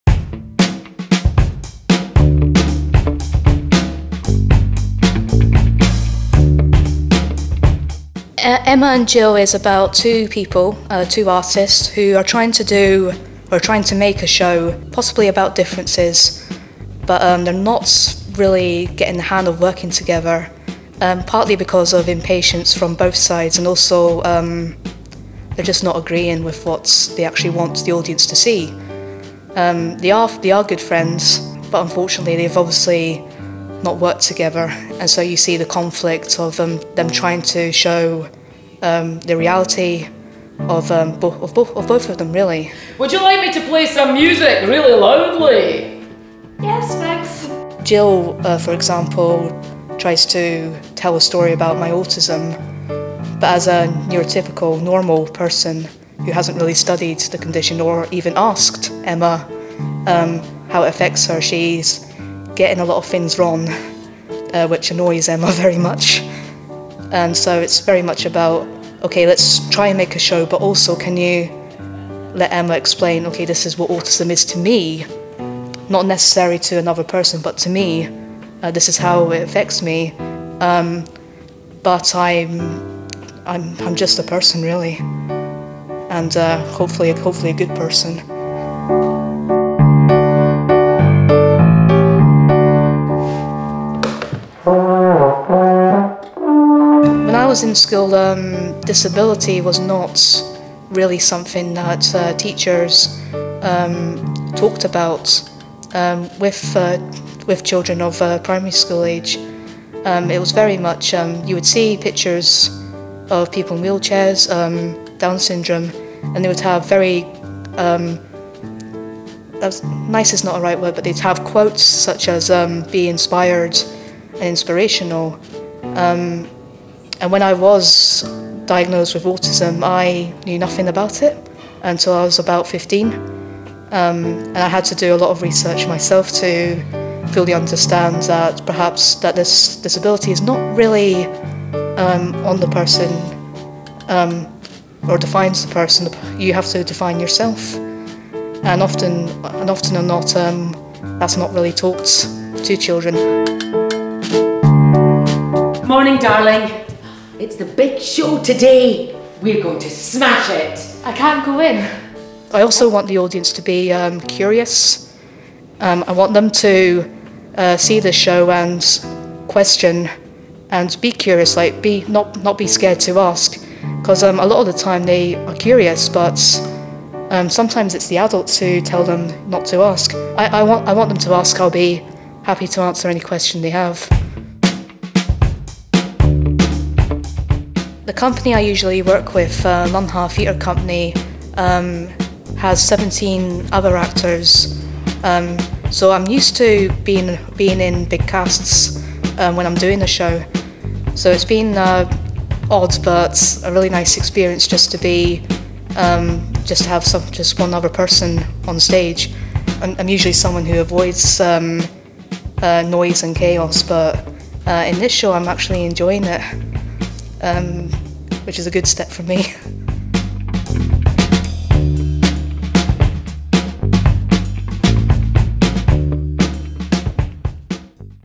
Emma & Gill | In rehearsals | Emma McCaffrey Interview
Emma & Gill tells the story of two friends who are making a theatre show together. The show is supposed to be all about Emma and her experience as an autistic person.

As the story unfolds, it becomes clear that Gill doesn’t know as much about Emma or autism as she thinks she does. Gill thinks Emma is a bit sad and tragic. She thinks that Emma is not normal – not like Gill and everyone else. She doesn’t realise that Emma has lots of hidden talents and interests, and that maybe they have more in common than Gill would think.

This new show for schools questions what is “normal” and why being different is to be celebrated.

Interviewee: Emma McCaffrey, actor, Lung Ha Theatre Company
Video: Sandy Butler Videography